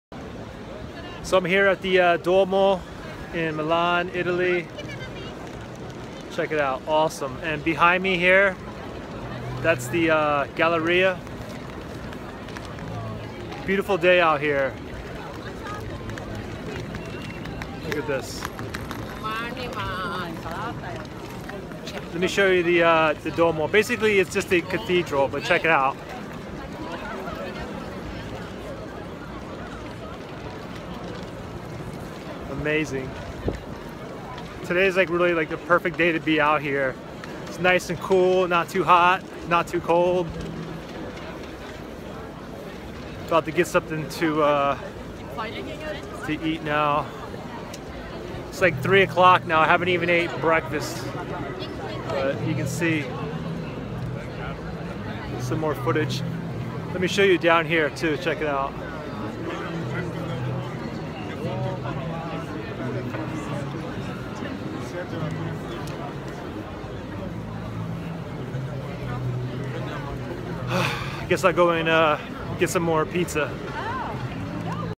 Vlog: At the Cathedral Church (Duomo) & Galleria in Milan Italy
This is just a simple blog using my iPhone 7 front facing camera. I'm in Milan at the Piazza del Duomo, next too the Duomo which is a gothic looking cathedral church and the Galleria. It is the fifth largest Christian church and took six centuries to build and it is still ongoing.